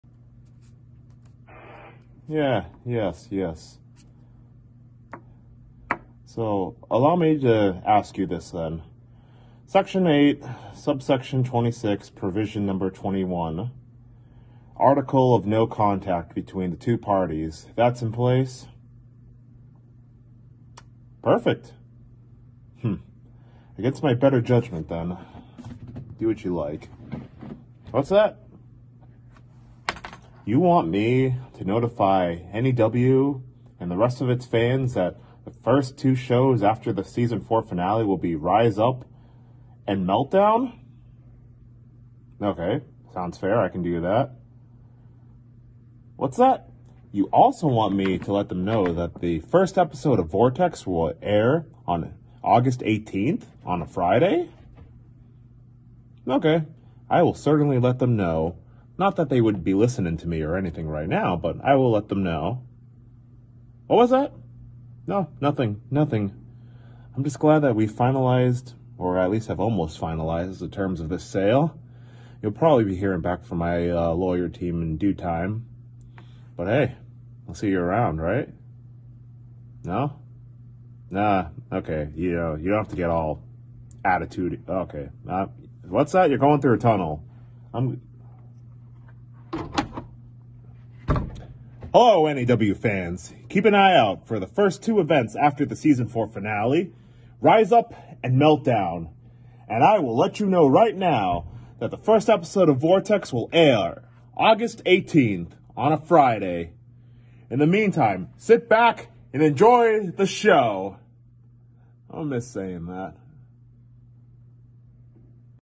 N.E.W: Season 5 Announcements
Justin Abel Sand has not only 1, not only 2, but THREE HUGE Announcements for Season 5!